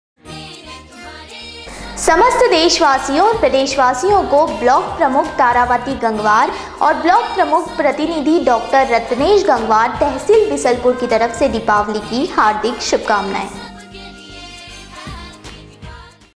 News29Indiaतारावती और डॉ रत्नेश गंगवार दीपावली की हार्दिक शुभकामनाएं।
ब्लॉक प्रमुख तारावती गंगवार और ब्लॉक प्रमुख प्रतिनिधि डॉ रत्नेश गंगवार तहसील बीसलपुर की तरफ से दीपावली की हार्दिक शुभकामनाएं।।